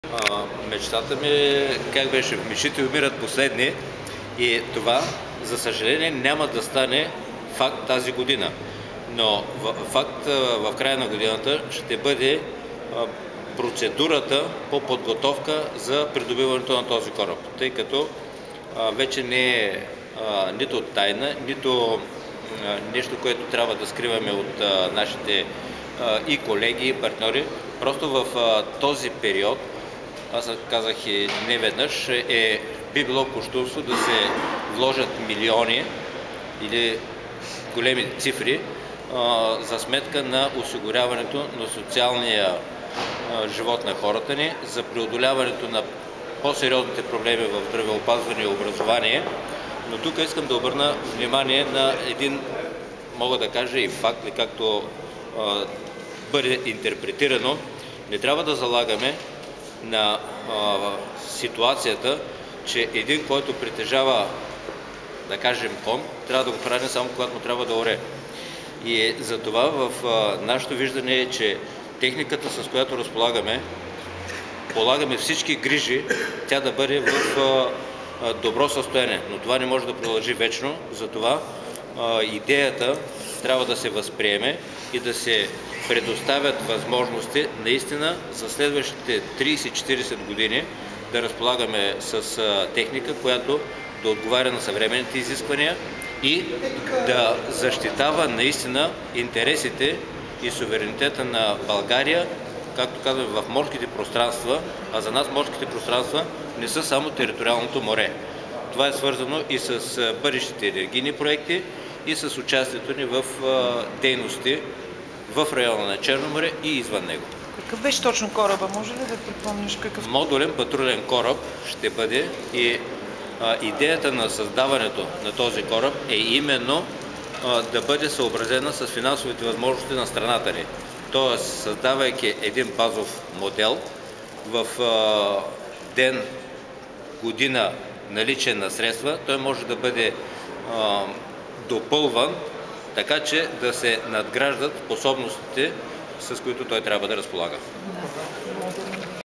контраадмирал Румен Николов - командващ ВМС
Няма пари за нов боен кораб на военноморския ни флот